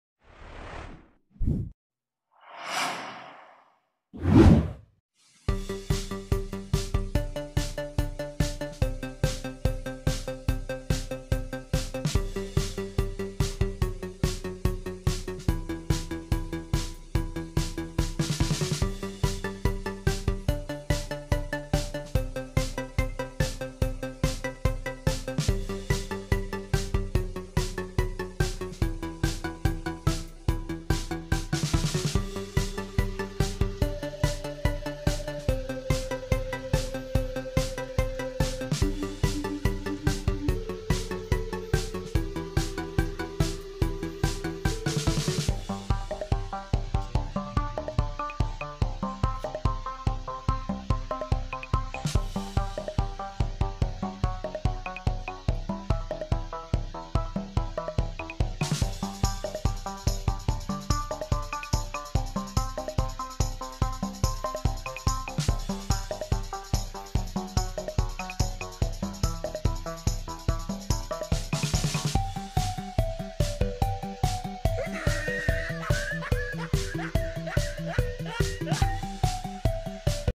Câu đố IQ - Di chuyển 2 que biến 5 hình hộp thành 4 hình/ moving 2 sticks turns 5 boxes into 4.
Giải đáp câu đố IQ hình hộp, di chuyển 2 que biến 5 hình hộp thành 4 hình.